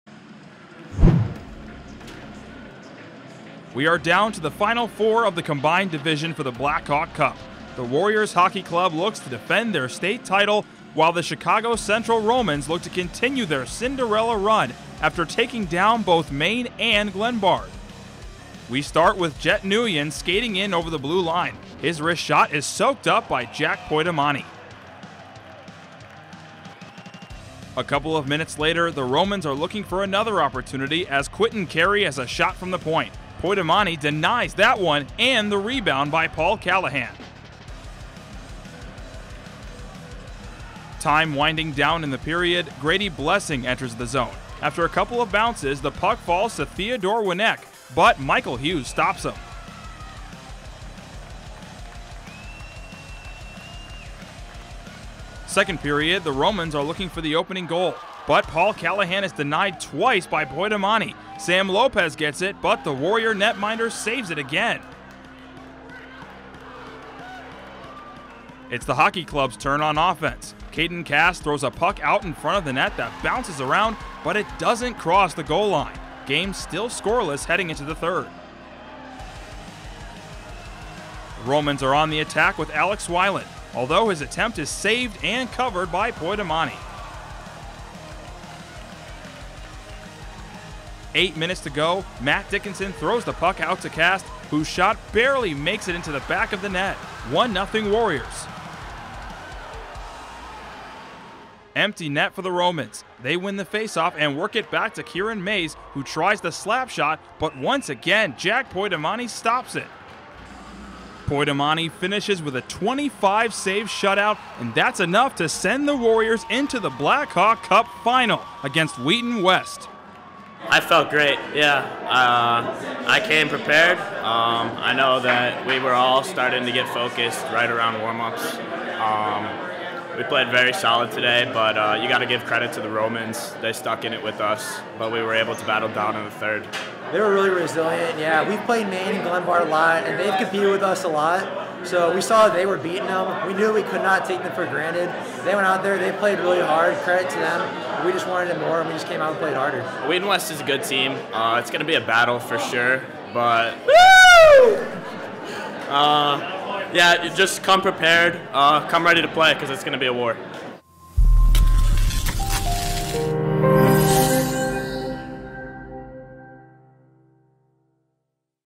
Hockey Blackhawk Cup Semi Final Chicago Romans vs. Warriors Hockey Club 03.10.22
The Warriors Hockey Club takes on the Chicago Romans Hockey Club in the Final Four of the Blackhawk Cup where the Warriors advance to the final once again.
We are down to the Final Four of the combined division for the Blackhawk Cup. The Warriors Hockey Club looks to defend their state title while the Chicago Central Romans look to continue their Cinderella run after taking down both Maine and Glenbard.

First Period

We start with Jet Nuygen skating in over the blue line. His wrist shot is soaked up by Jack Poidomani.

A couple of minutes later, the Romans are looking for another opportunity as Quinten Carey has a shot from the point. Poidomani denies that one and the rebound by Paul Callahan.

Time winding down in the period, Grady Blessing enters the zone. After a couple of bounces, the puck falls to Theodor Wnek, but Michael Hughes stops him.

Second Period

Second period, the Romans are looking for the opening goal, but Paul Callahan is denied twice by Poidomani. Sam Lopez gets it, but the Warrior netminder saves it again.

It’s the Hockey Club’s turn on offense. Caden Cast throws a puck out in front of the net that bounces around, but it doesn’t cross the goal line. Game still scoreless heading into the third.

Third Period

The Romans are on the attack with Alex Wilen. Although, his attempt is saved and covered by Poidomani.

Eight minutes to go, Matt Dickinson throws the puck out to Cast whose shot barely makes it into the back of the net. 1-0 Warriors.

Empty net for the Romans. They win the faceoff and work it back to Kieran Mayes who tries the slap shot, but once again, Jack Poidomani stops it.

Poidomani finishes with a 25-save shutout and that’s enough to send the Warriors into the Blackhawk Cup final against Wheaton West.